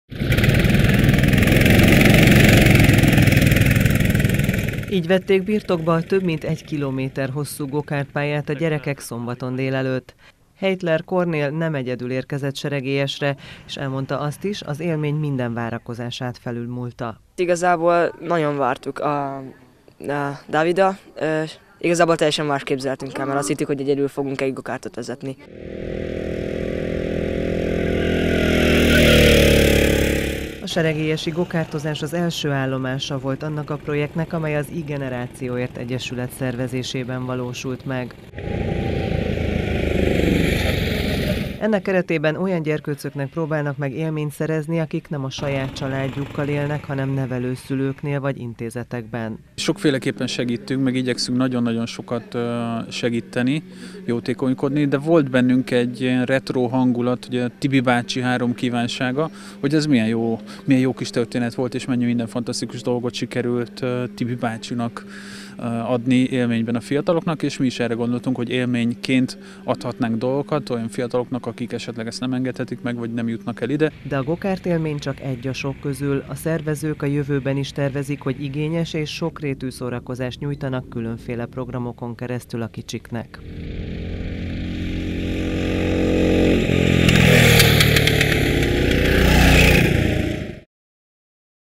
ÉLMÉNY MINDENKINEK
Gokartoktól volt hangos Seregélyes szombaton délelőtt. Ezúttal nevelőszülőknél és intézetekben élő gyerkőcök érkeztek a pályára, a Bakancslista projekt első állomására, amely az iGenerációért Egyesület szervezésében valósult meg.